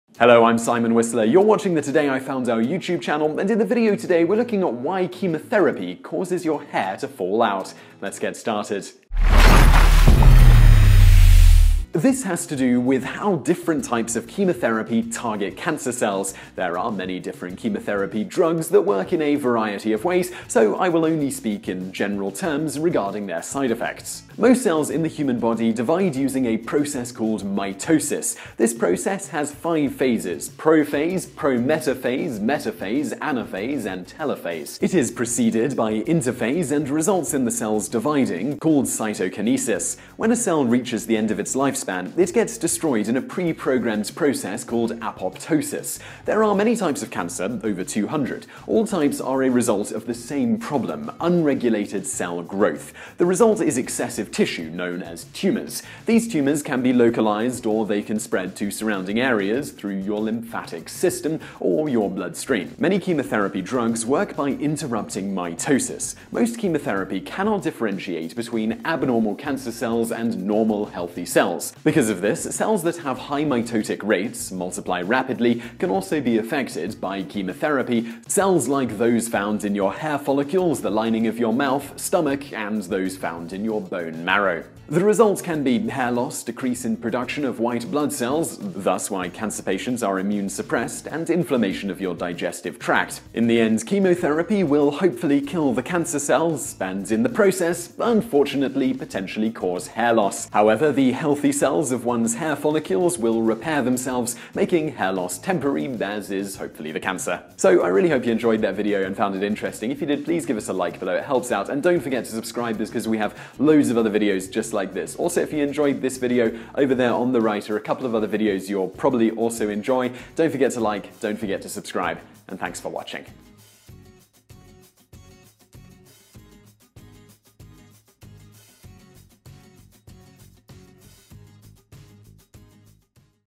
Why Chemotherapy Cause Hair to Fall Out
In this video:

Maya I love the question. It seems like once or twice a year I’m shaving my head to help raise money for cancer research and at every fundraiser, the question seems to come up- Why does the common treatment for cancer make body hair fall out? The answer is really pretty simple. It has to do with how different types of Chemotherapy target cancer cells. There are many different Chemotherapy drugs that work in different ways, so I will only speak in general terms regarding their side effects.